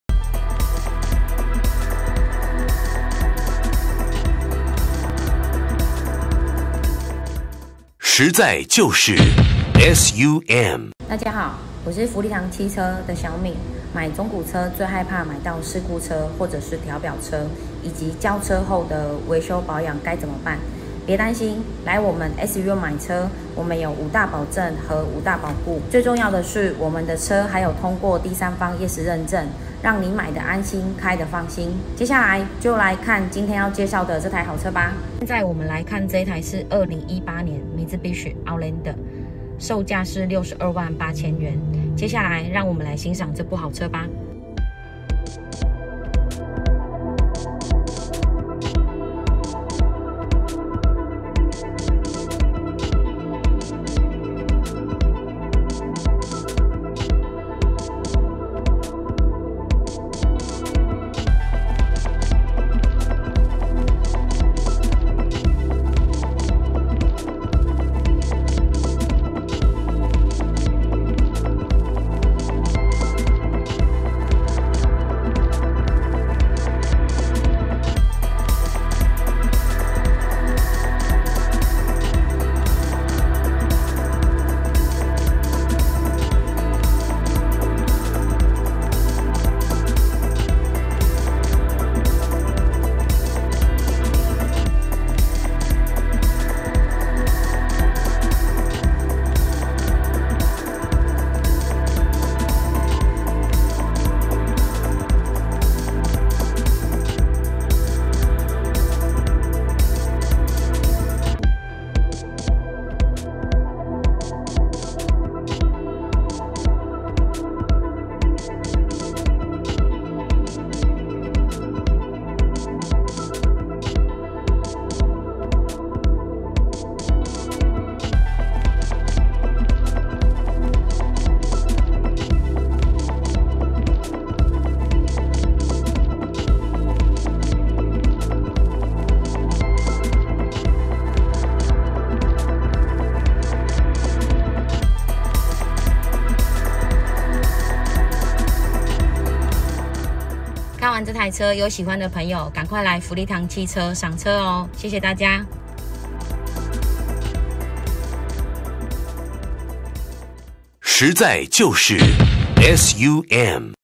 【SUM中古車】MITSUBISHI OUTLANDER 主動真安全 高CP值 2018年 台南市
車商資訊：SUM福利堂汽車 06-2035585
連絡資訊：潘自仲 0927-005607 LINE: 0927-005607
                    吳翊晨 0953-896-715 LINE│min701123
車輛資訊：2018年MITSUBISHI OUTLANDER 原鈑件/ACC跟車/免鑰匙/AEB緊急煞停/盲點/360環景/電動尾門
車商地址：台南市永康區中山北路993之1號